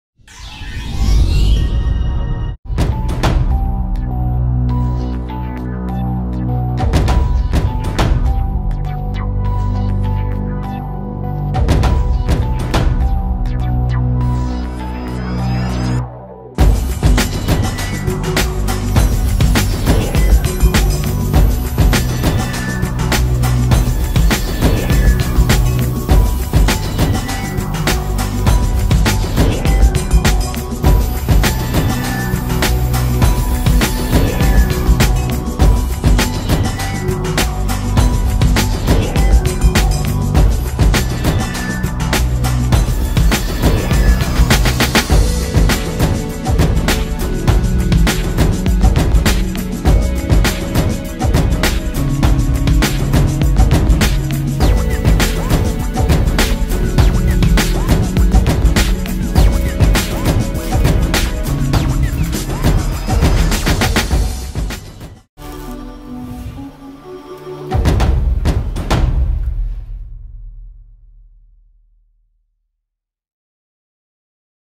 TAF TOOL - Demo
We have worked over the year on a software tool that would help our clients to easily build up their own stand or show design from aluminium trusses. It seems that the long -term work paid off and our TAF TOOL software is going to be released within few weeks.